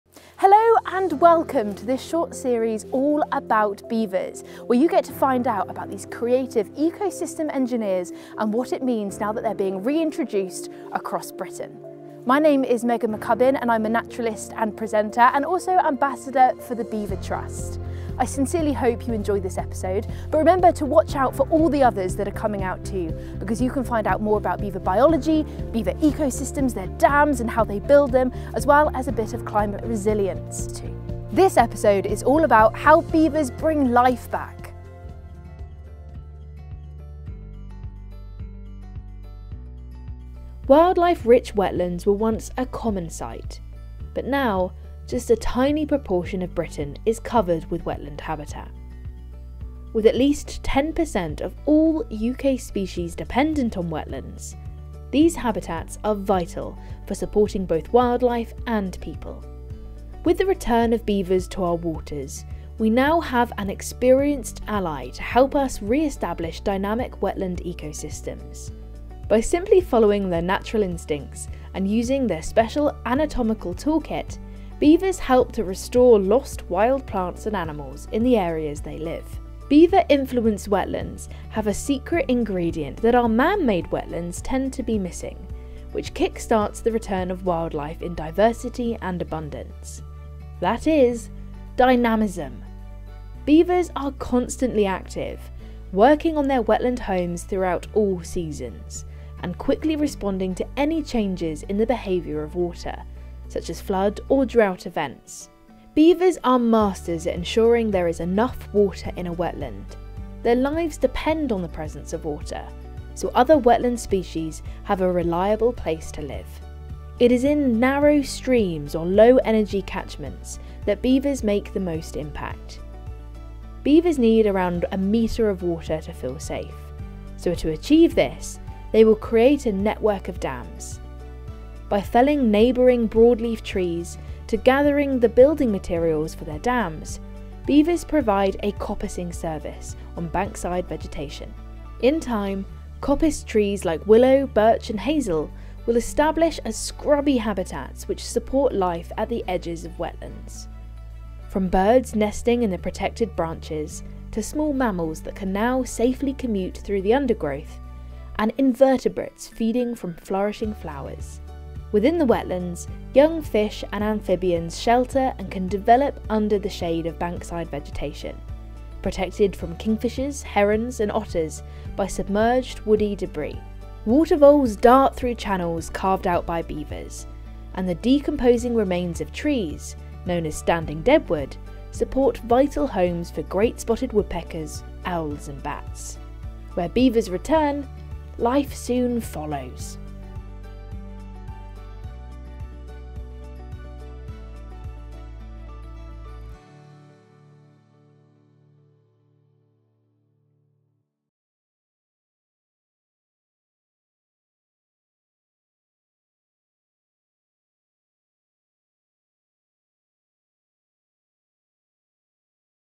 Beavers: How they bring life
Discover how beavers can breathe life into our landscapes and help us to rewild.

By simply following their natural instincts, beavers help restore lost wetland habitats and welcome back the species that rely on them.

We're delighted to have worked with @wildintrigue to produce these videos.

We envision this series being used widely for a variety of audiences across different host platforms from school lesson starters accompanying a lesson pack, community hall screenings and more.